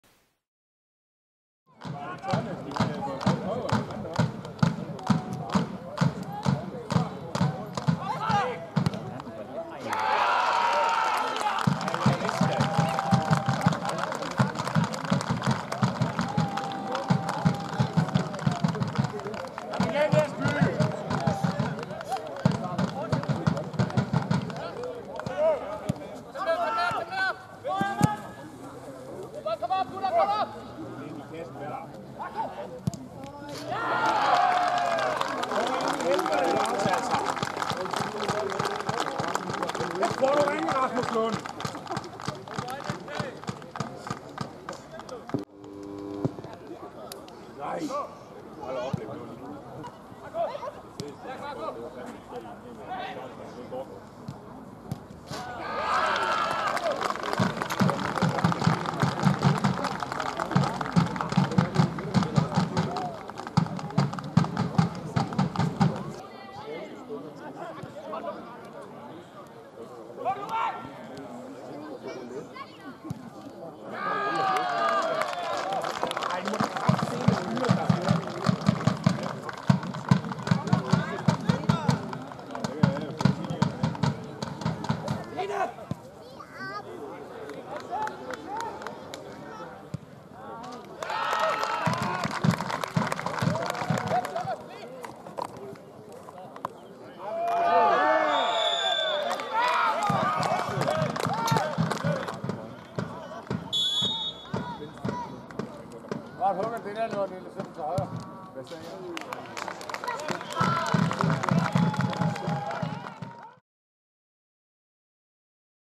FCF TV - Se målene fra 6-0 sejren i pokalen
FC Fredericia spillede sig sikkert videre fra første runde af DBU Pokalen. Se eller gense målene i 6-0 sejren her.